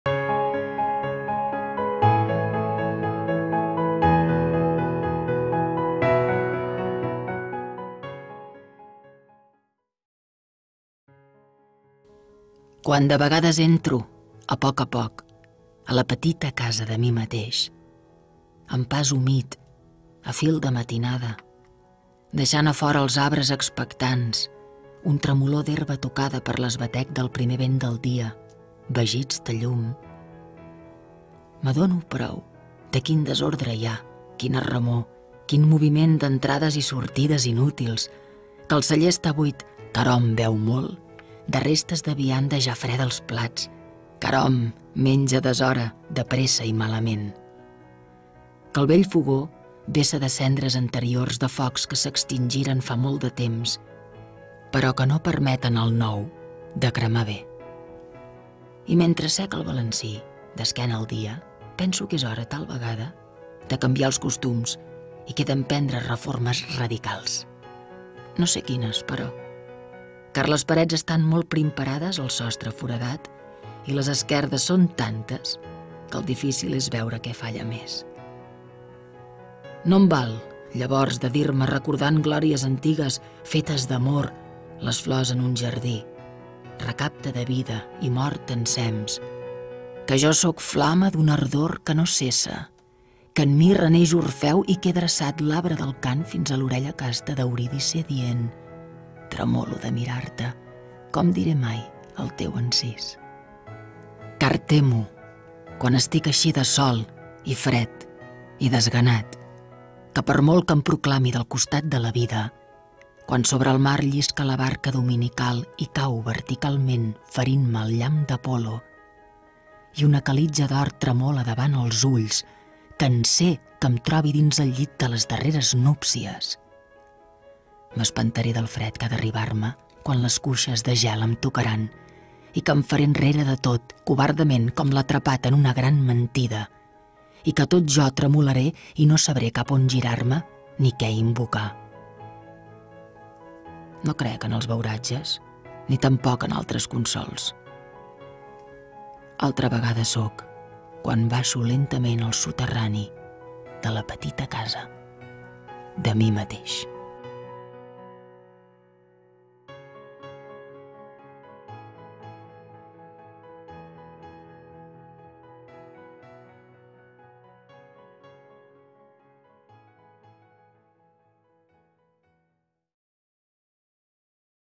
"L'acte darrer", de Joan Vinyoli [projecte Poesia Dibuixada]
Impulsat per celebrar els 15 anys de LletrA (Universitat Oberta de Catalunya) com una col·laboració de la UOC i Catorze, POESIA DIBUIXADA és un projecte literari i multimèdia on l’art, la poesia i l’audiovisual es fusionen per oferir una experiència lectora innovadora a partir de la creació de breus obres d’art audiovisuals. Cadascuna de les poesies dibuixades integra un poema d’un autor o autora clàssic o contemporani, la veu de Sílvia Bel i les il·lustracions de Marta Bellvehí, que hi posa el color amb pinzellades d’aquarel·la i línies a llapis.

Poesia dibuixada amb un poema de Joan Vinyoli.